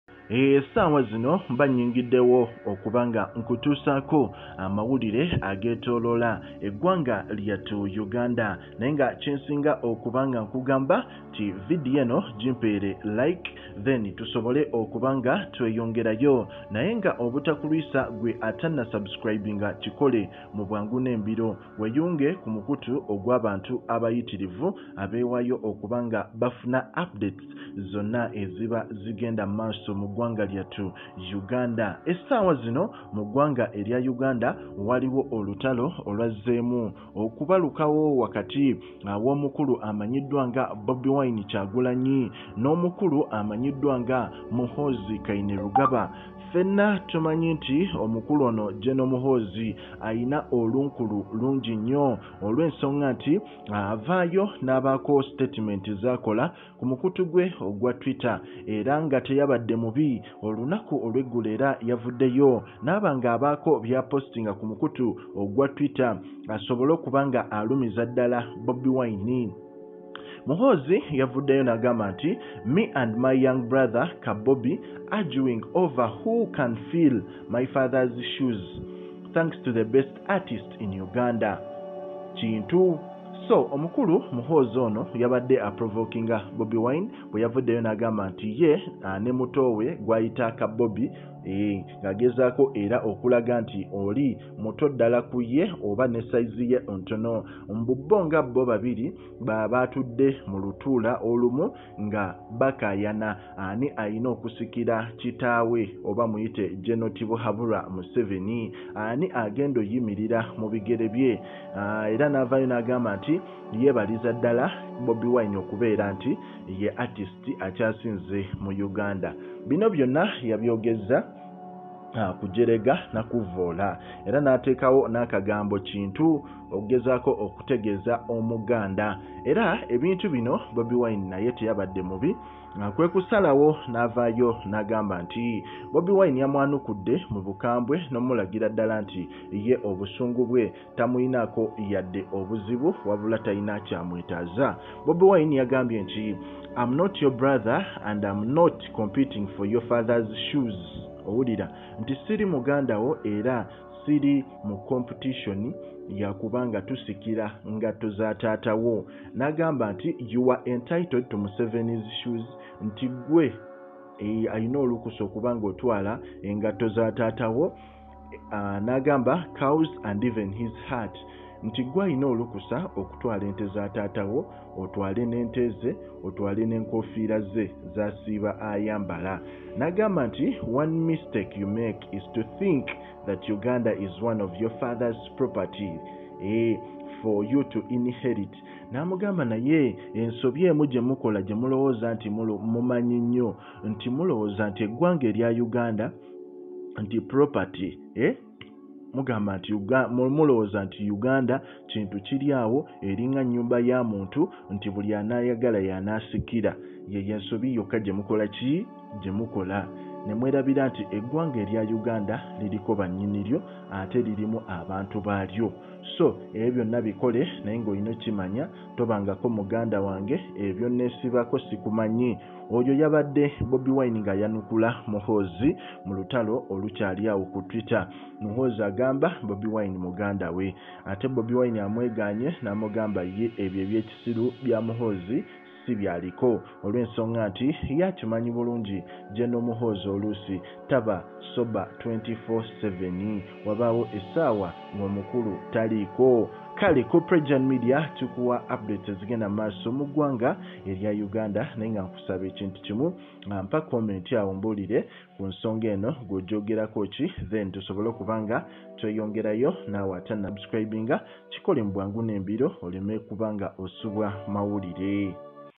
Olutalo lutandise! Bobi wine Alabudde muhoozi Tolina gwogenda kufuggaa mu Uganda ate Siri muganda wo
Firebrand politician and National Unity Platform principal Robert Kyagulanyi Ssentamu has hit at president Museveni’s son and commander Land Forces Muhoozi Kainerugaba over a tweet gone wrong
Muhoozi who has been moving around together with his supporters in different regions popularizing his presidential ambitions in the next general election now sees Bobi Wine as his competitor to his father’s throne.

In his usual tweets, Muhoozi shared a cartoon of him fighting with Bobi Wine over the dusty throne. This didn’t go well with the man from Magere who hit back with a recharged mood.

In a veiled message that sounded like a warning to the ‘tweeting’ Army General. Bobi Wine blasted Muhoozi for thinking that Uganda is his father’s property he’s struggling to inherit. He also assured him that he’s not Museveni’s son to compete for his shoes.